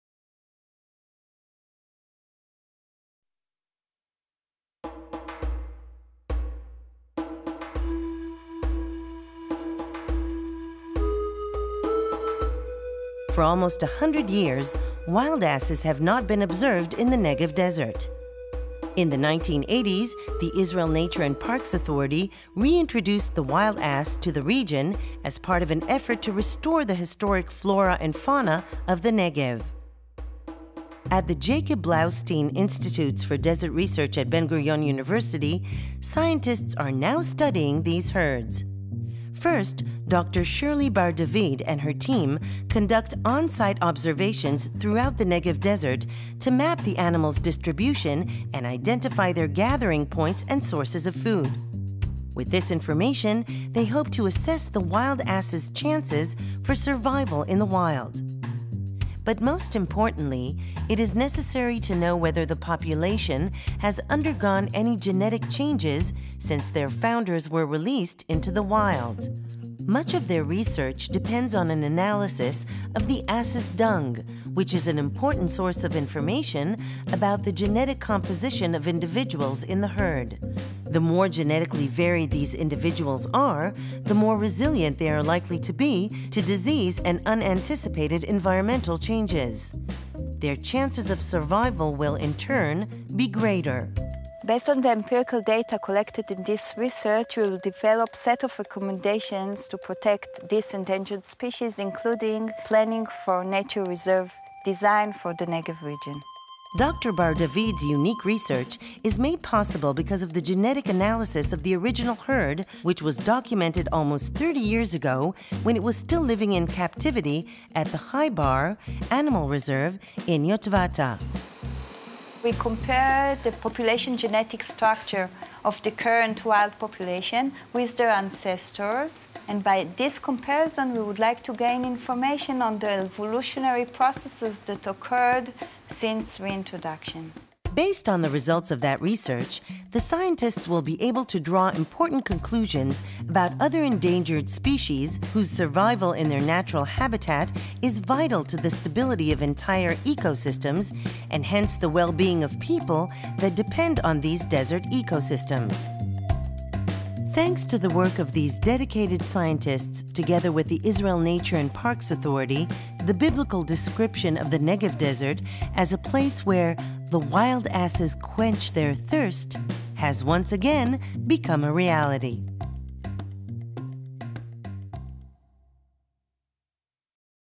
Wild Ass genetic research.VOB
Genetic research of the reintroduced Wild Ass population (Equus hemionus) in Shirli Bar-David's Lab.